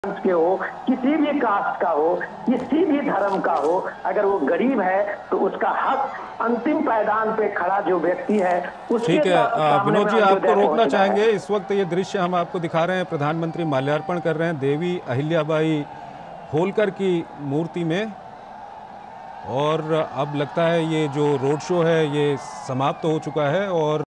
PM Modi pays floral tribute to Devi Ahilyabai in Indore, Madhya Pradesh
Prime Minister Narendra Modi paid floral tributes to Devi Ahilyabai Holkar in Indore, Madhya Pradesh.